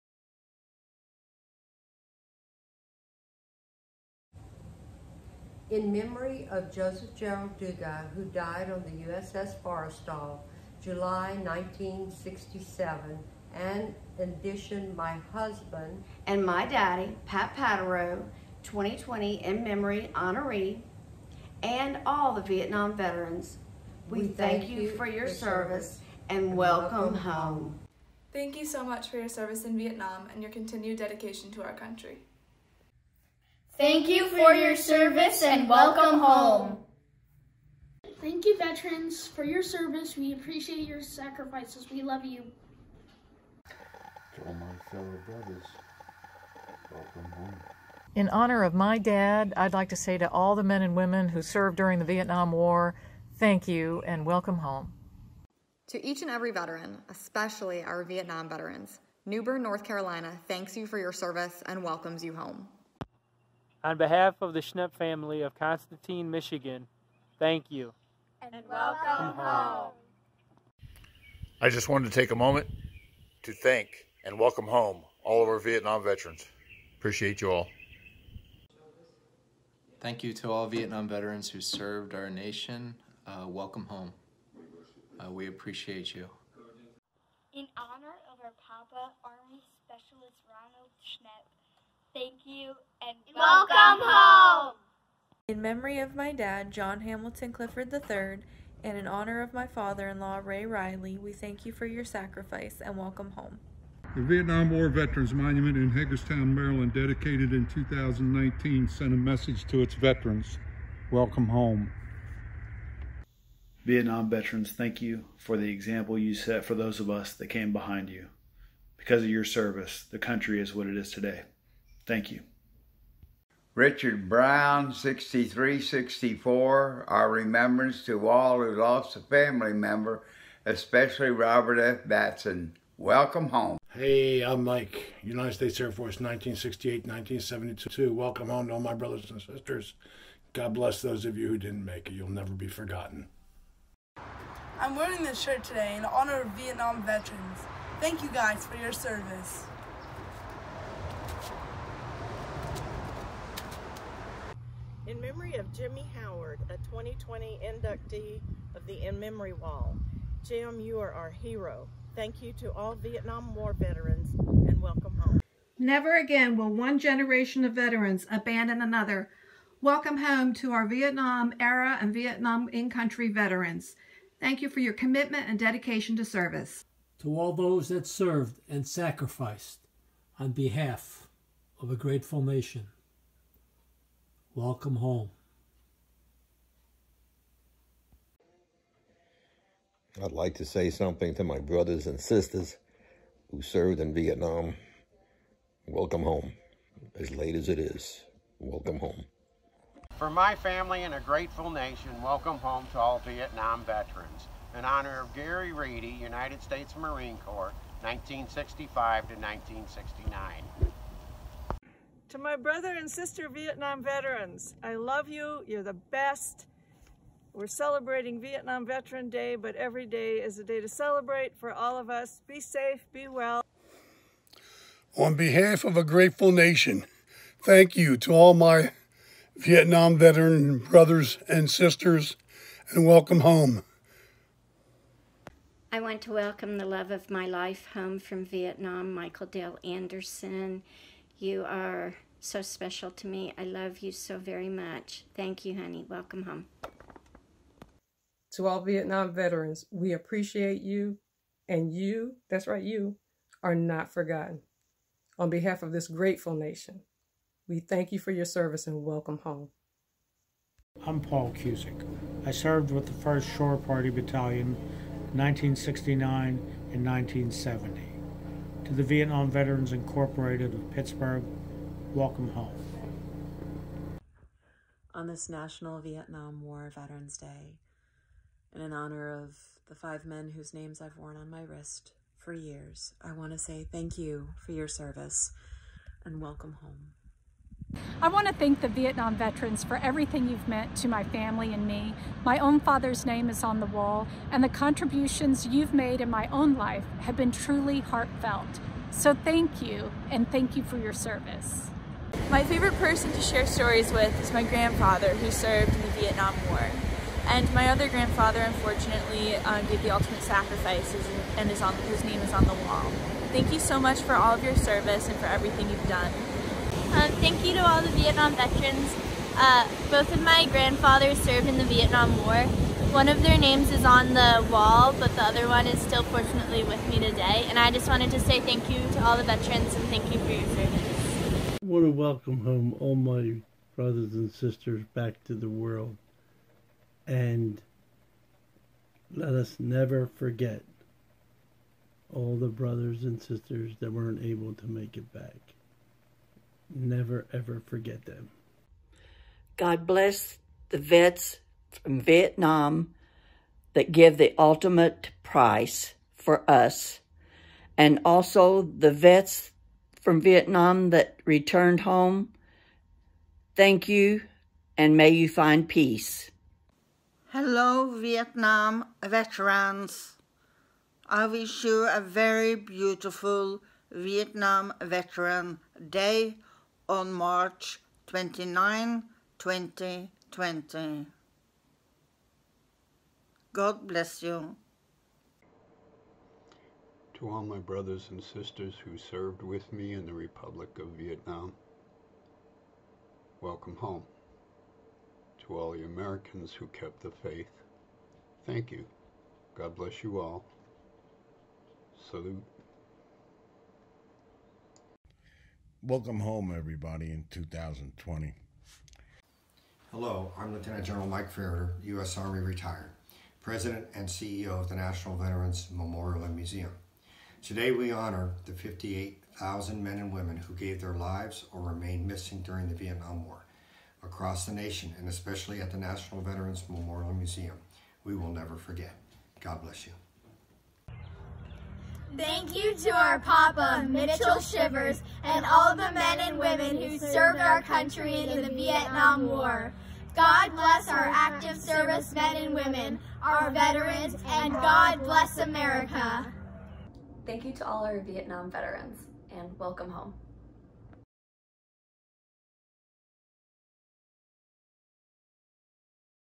"Welcome Home" - Your Messages to our Vietnam veterans
People from all over the world shared with us their special messages to thank and welcome home our Vietnam and Vietnam Era veterans.